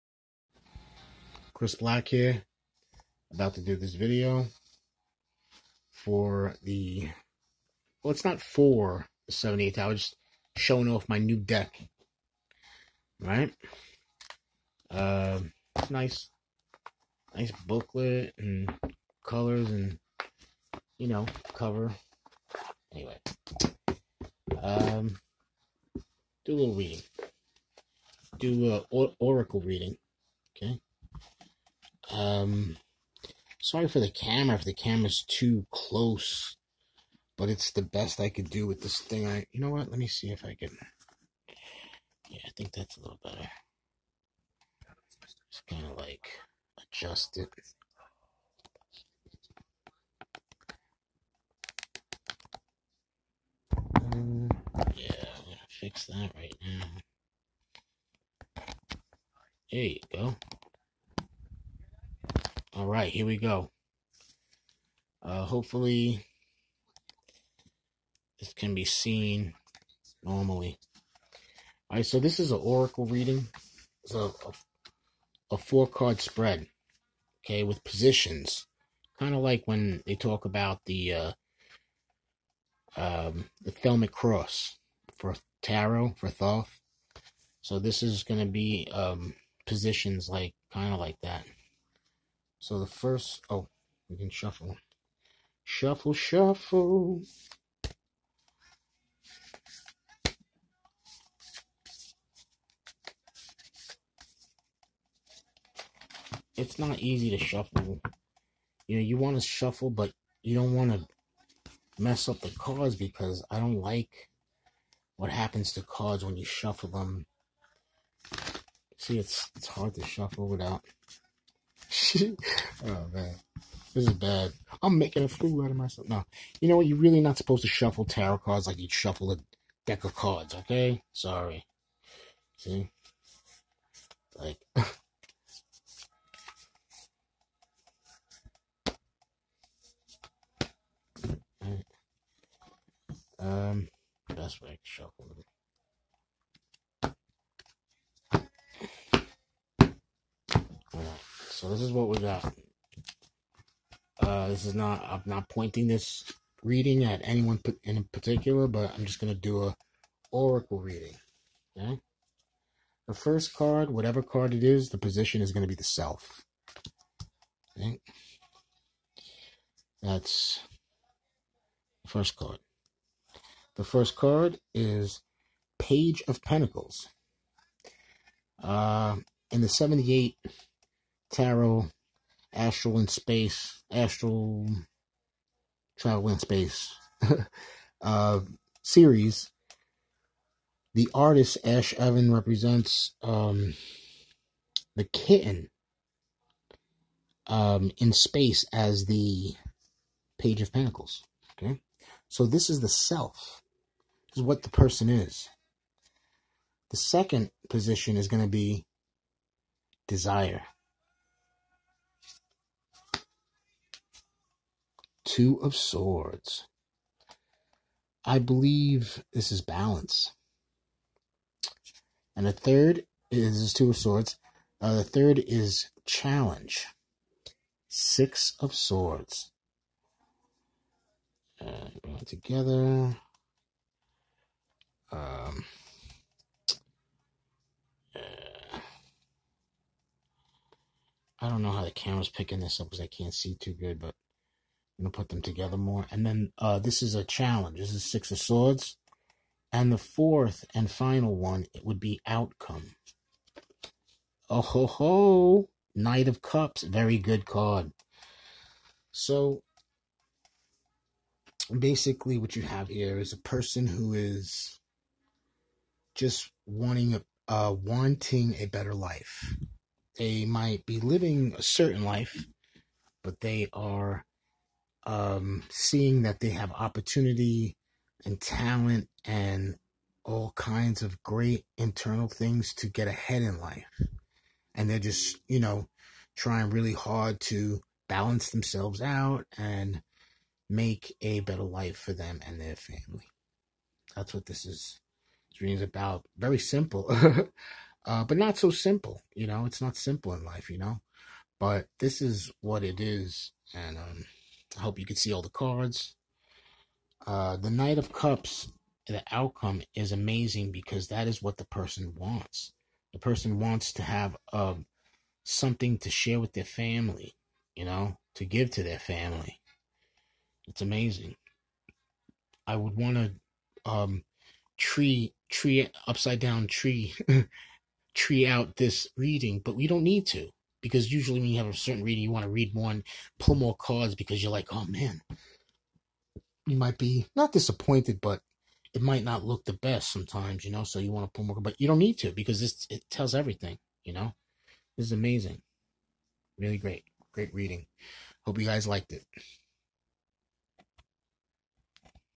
Oracle Spread Using 78 Tarot Deck - ATS
Decided to put away the Santa Muerte Deck I've been using for years. Now I busted out with the new fresh 78 Tarot - Astral Travel in Space. Love this deck and you can see the simple Oracle Spread here on this video with positions which are the most important for this oracle type of reading.